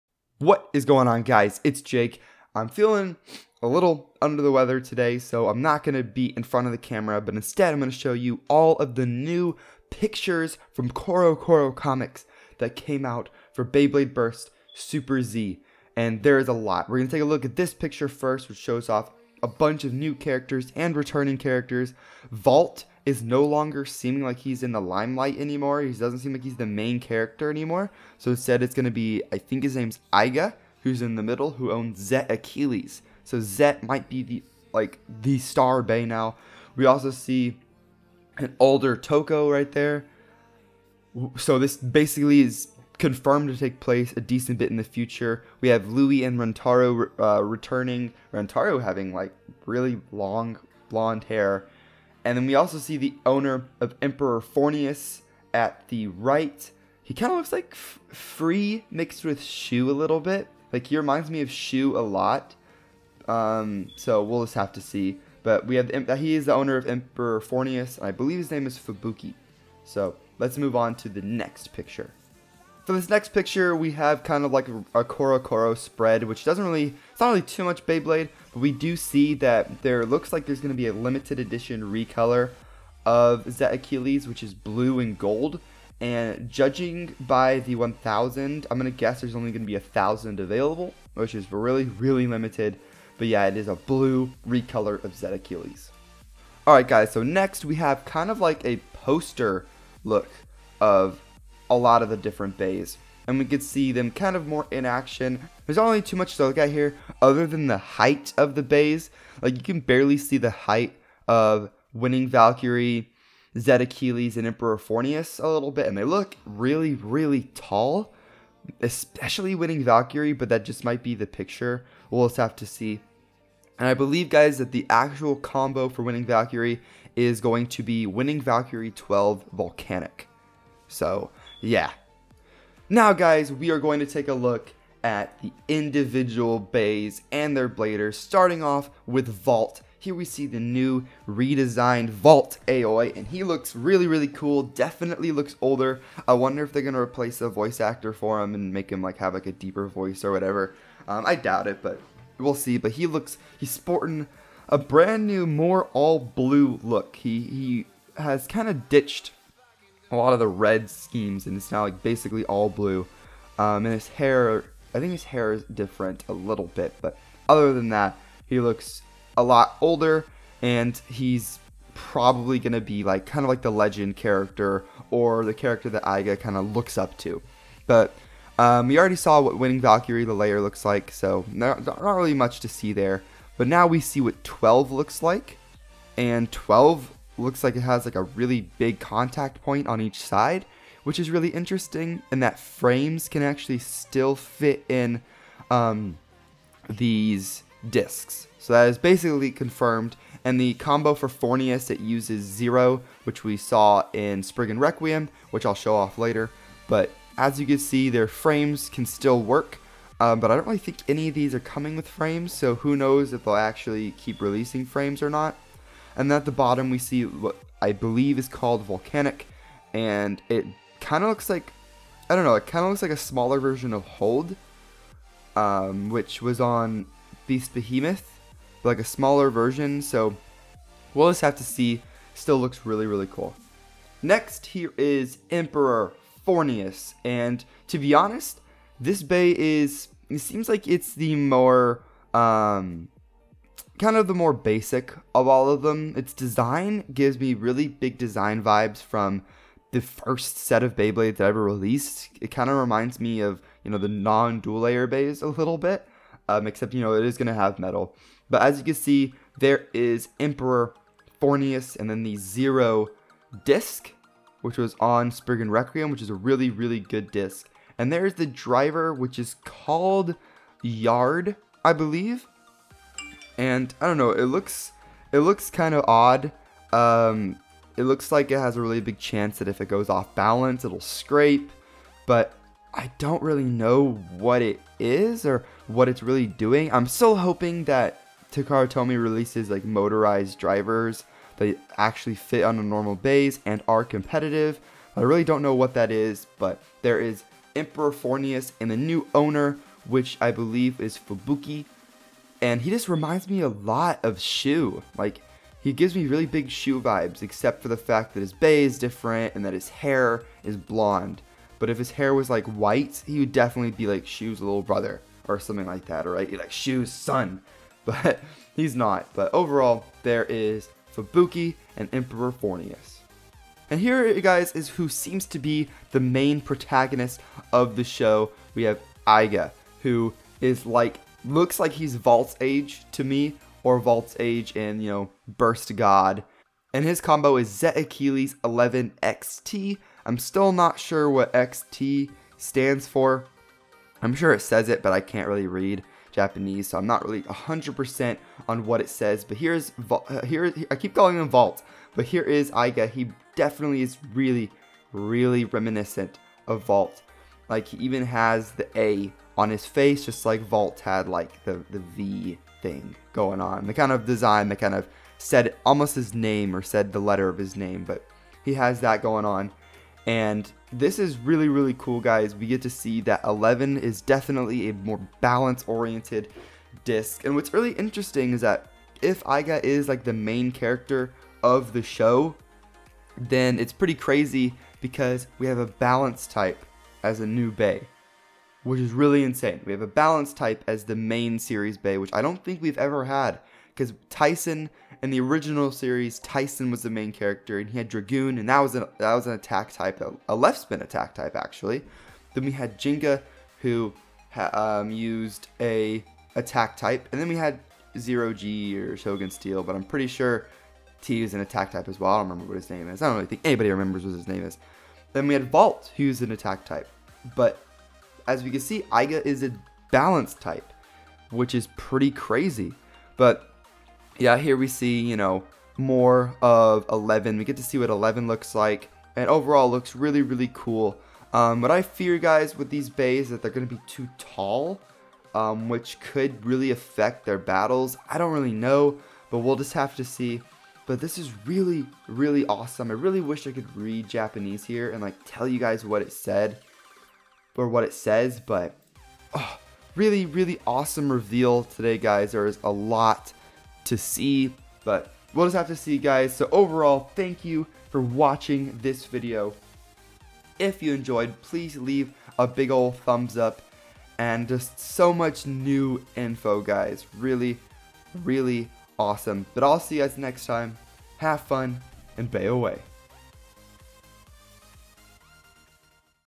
*NEW* Beyblade Burst Super Z (Chouzetsu) INFO!! || Beyblade Burst News and Info
YOOO, SO CoroCoro Comic released more info about the next series of Beyblade!! Super Z!! This time we see that VALT may not be the main Character!!!